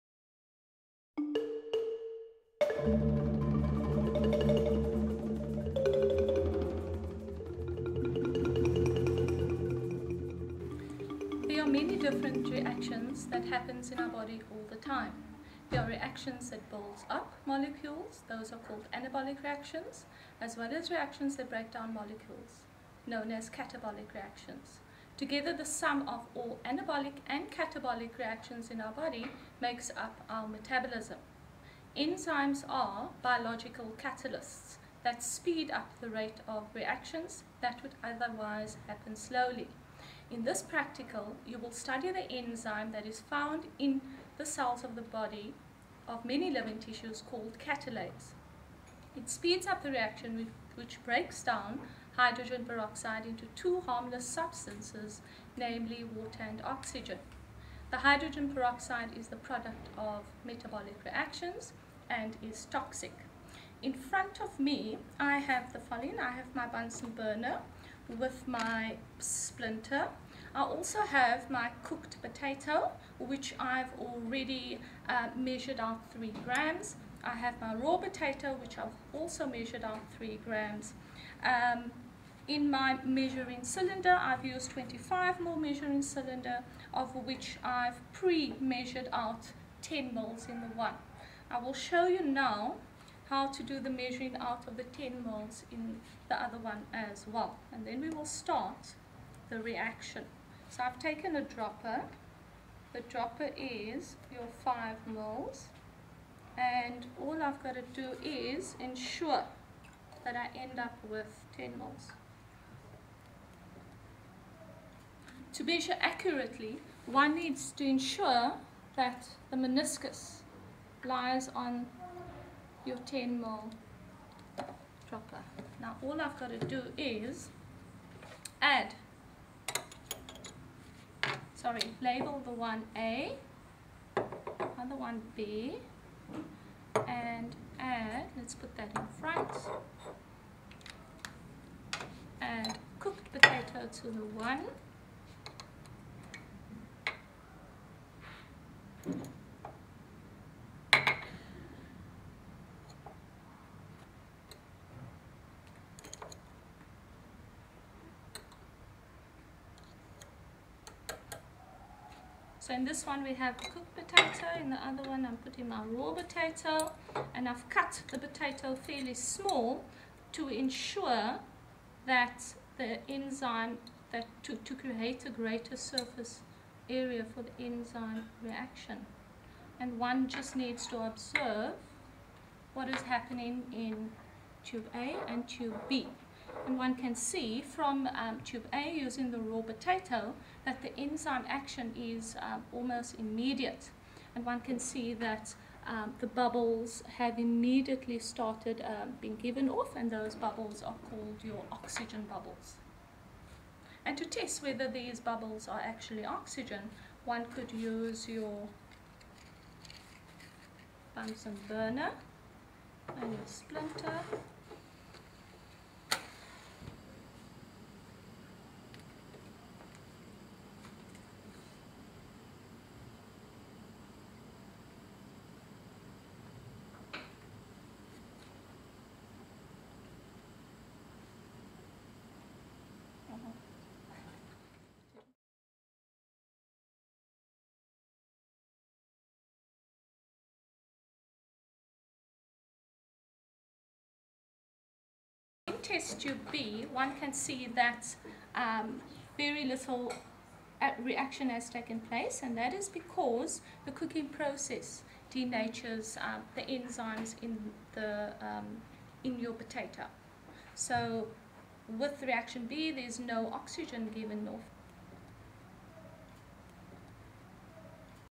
Video 16 CATALASE IN POTATOES
A Grade 10 Life Sciences practical using raw and cooked potatoes to demonstrate how catalase in the potatoes helps to speed up the breaking down of hydrogen peroxide into oxygen and water.